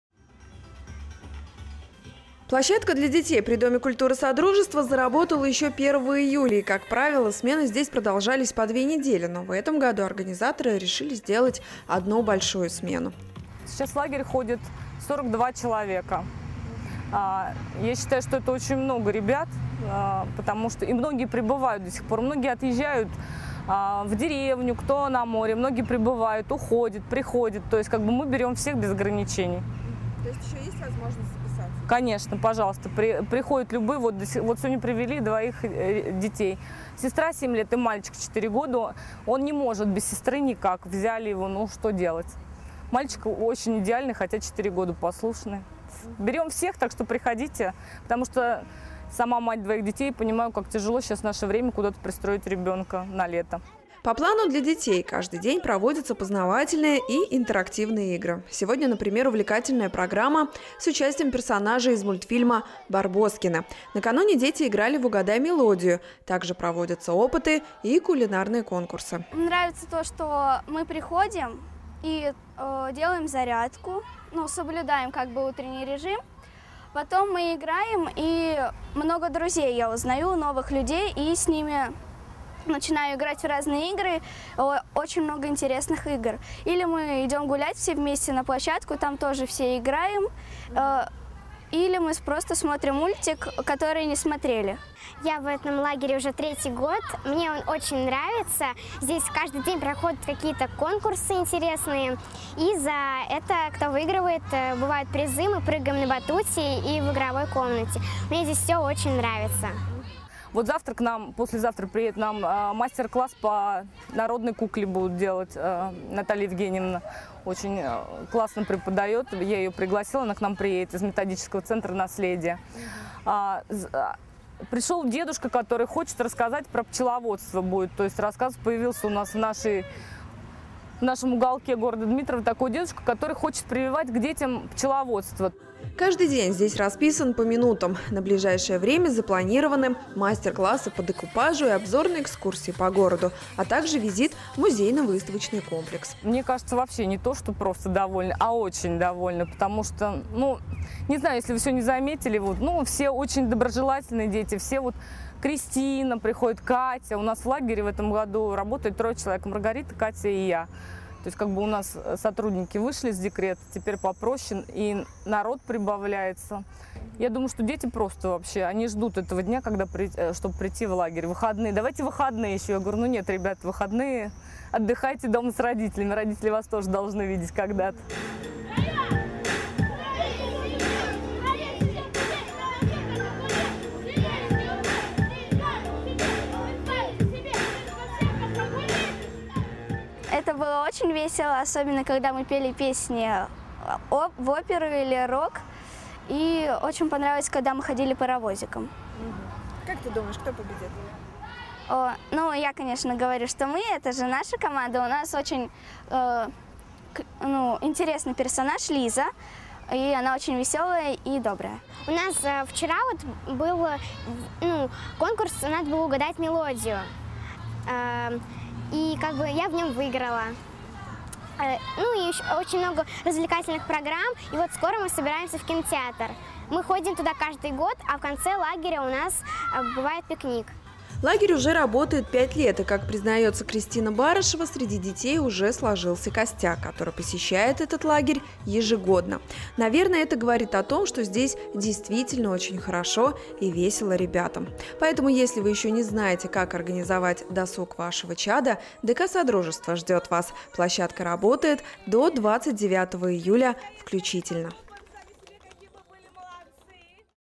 Детский лагерь в ДК "Содружество"
Время  летних каникул в самом разгаре. И когда если не сейчас, задумываться об организации  детского отдыха. Об этой теме мы не раз рассказывали в наших сюжетах, на днях  наша съемочная группа отправилась в ДК Содружество поселка ДЗФС